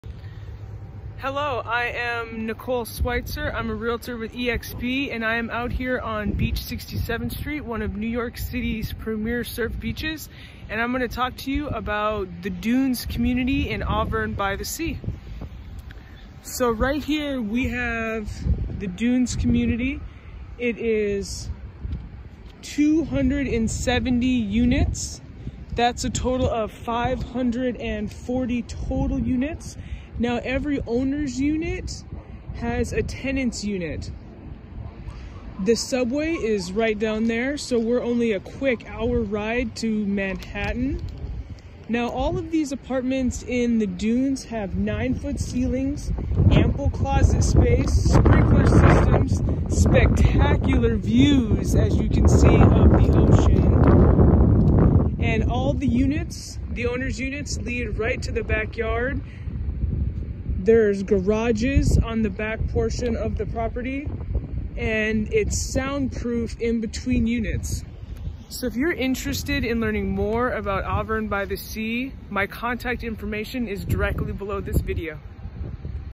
The Dunes at Arverne by the Sea - Homes for Sale and for Rent
This video is about the Dunes Community in Arverne by the Sea.
Homes for sale range from 1,000,000 - 1,500,000

Great Community
- 20-year tax abatement and income-producing rental apartment
 - 9-foot ceilings throughout first floor
– Grand entry foyer
– Luxurious master suites
– Ample closet and storage space
– Oak railings on stairs
– Raised panel colonial interior doors throughout
– Brass finished level handles and colonial wood trim on doors
– Plush wall to wall carpet throughout with choice of designer colors
– Large terrace level storage room or den, per plan
– Fire sprinkler for your protection
- Spectacular private roof terraces (on selected end unit locations as per plan)
– Steel front entry door with side glass lights on owner’s home
– Rear exterior doors leading to owner’s yard
– Private owner’s garage (except the Commodore)
– Distinctive siding and trim exteriors
– Aluminum gutters and leaders
– 40 year architecturally dimensional Owens-Corning roof shingles
– Full landscaped front yards
– Sound proofing between homes
– Double glazed low E vinyl constructed windows
– Exterior rails constructed from maintenance-free polyvinyl

Nicole Switzer
Licensed Real Estate Agent